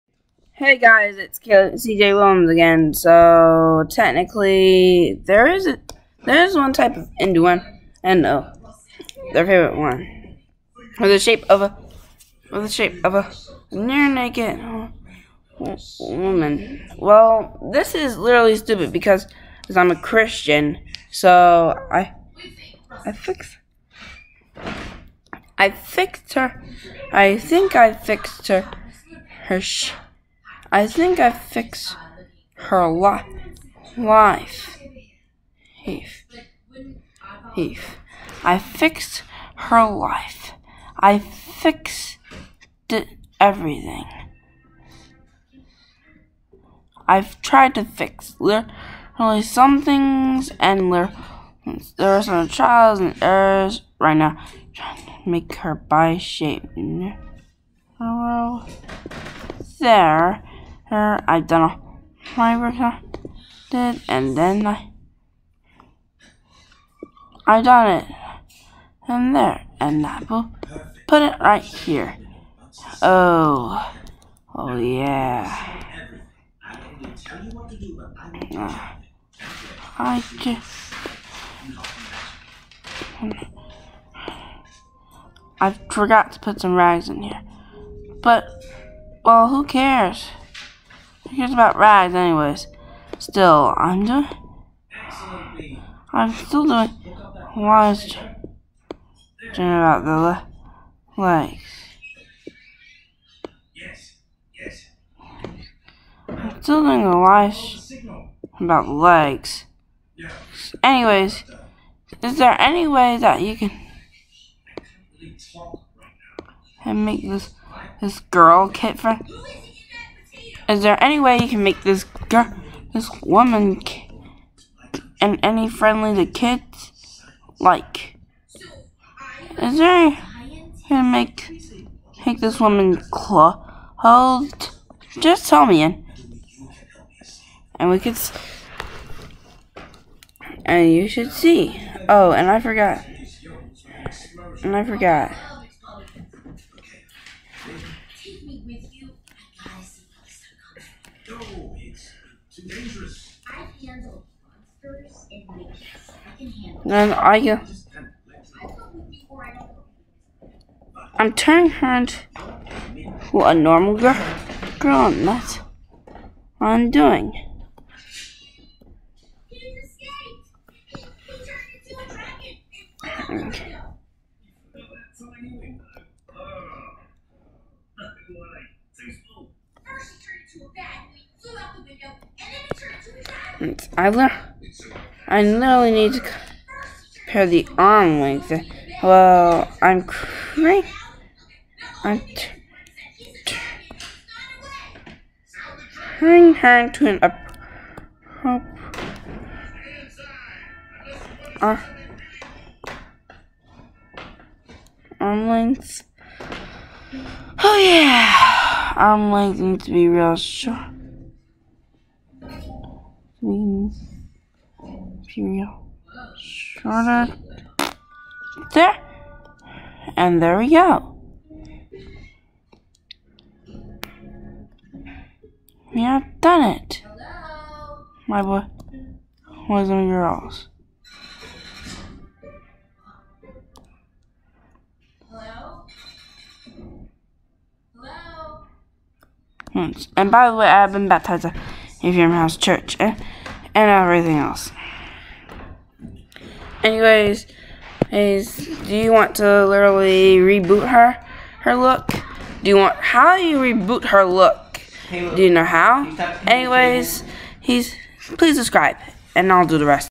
I clothed her
She now has a page in the Big Chungus wiki
That's right! I extended the body of this *. And I even clothed her blinding body. Is there any way to redraw her? If so, use InduenoMustGo to fix her half-naked sus.
"the shape of a near-naked woman appeared on The King Has Returned poster. There’s no denying this one, Disney."
-Metro.co